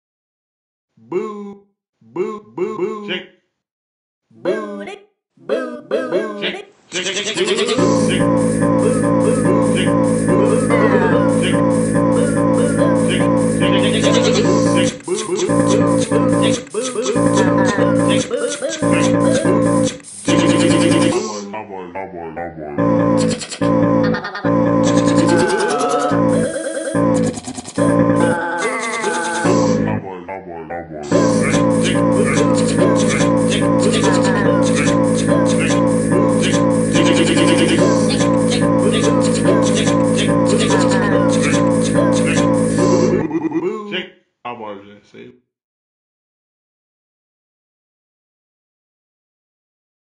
Booty Shake Vid Rhythm
Hahaha funny video of my fiancé on vid rhythm